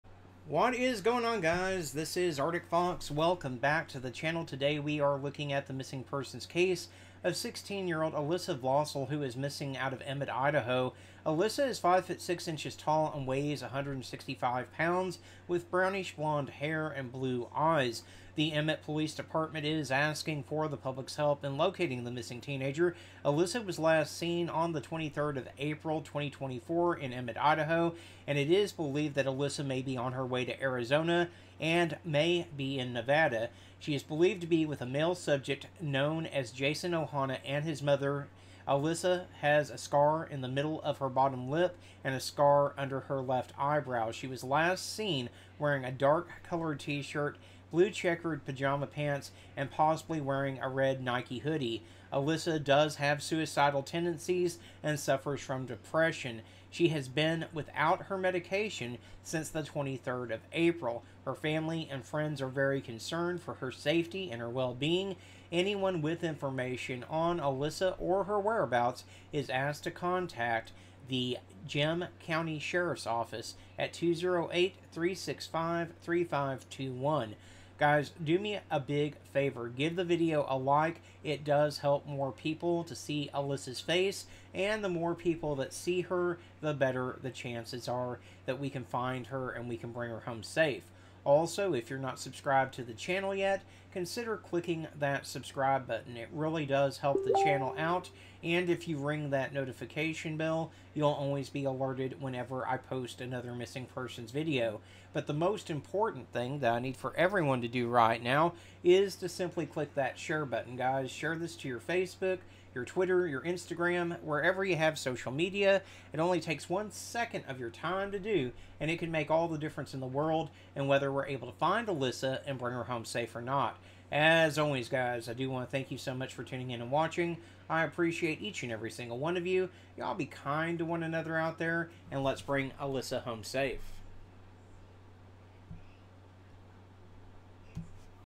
16 YEAR OLD ALYSSA VLACIL IS MISSING FROM EMMETT IDAHO. SHARE HER FACE TO BRING HER HOME SAFE!!!
ALCOHOLICS ANONYMOUS